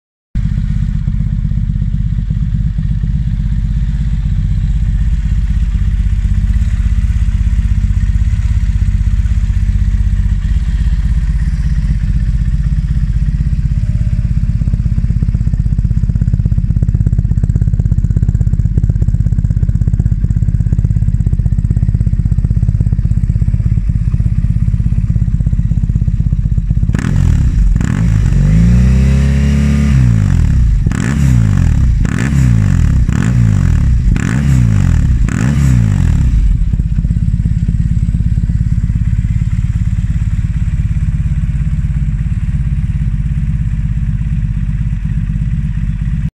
COMPLETE 2-1 EXHAUST SYSTEM GR FOR YAMAHA XTZ750 SÚPERTENERÉ
Complete exhaust system GR 2 into 1 + 1 eliptical silencer. Matte finished and welds at sight.